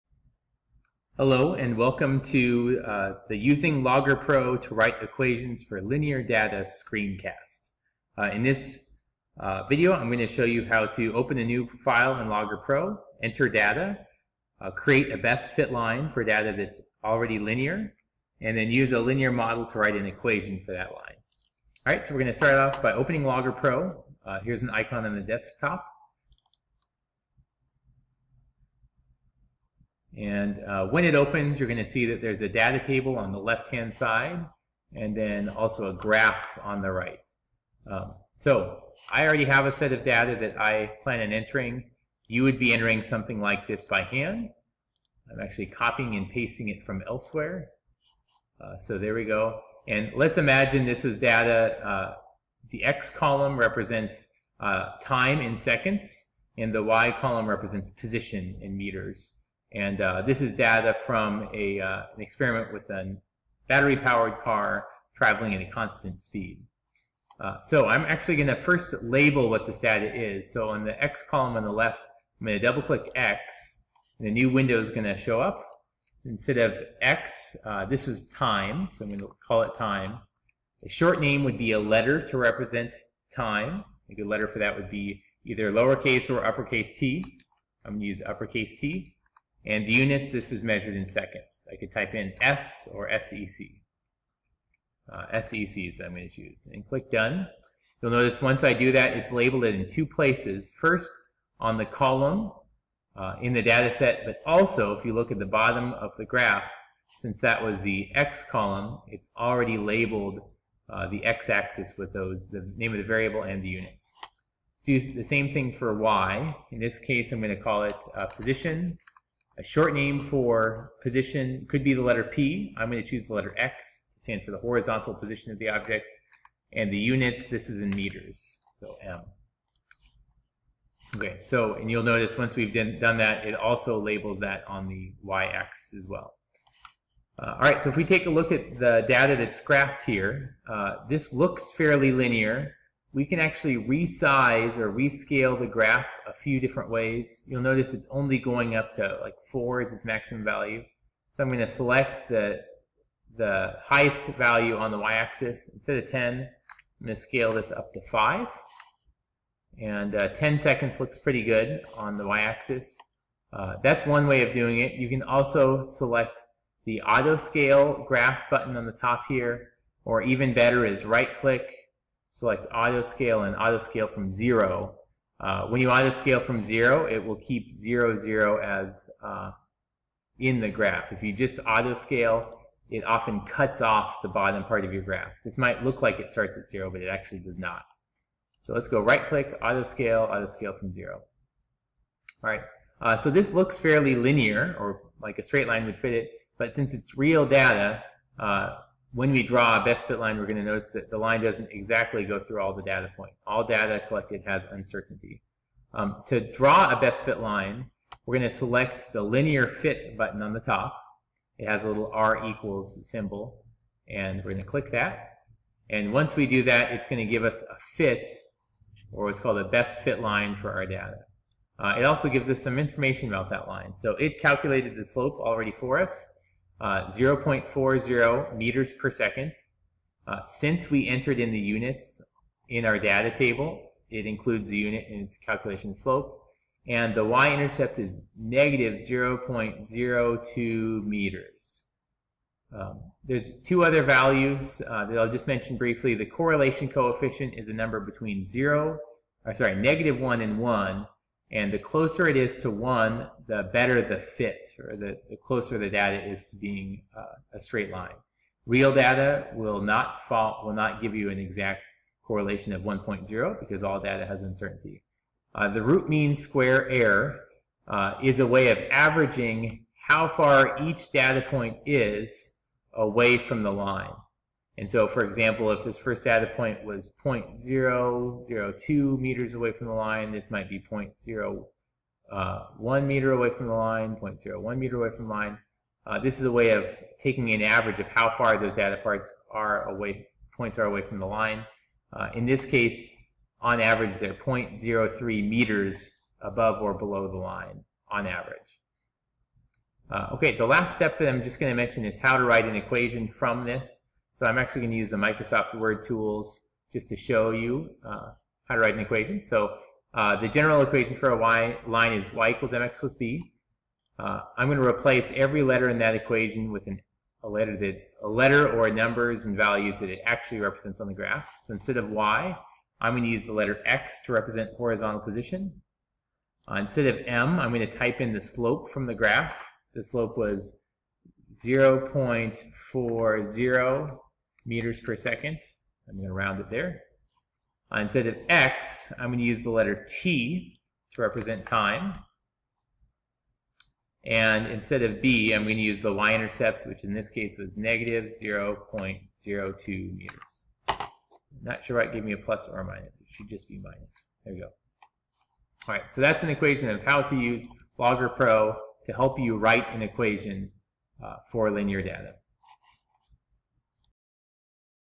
LoggerPro with Linear Data
How to use LoggerPro to graph data and then write an equation for data that is linear.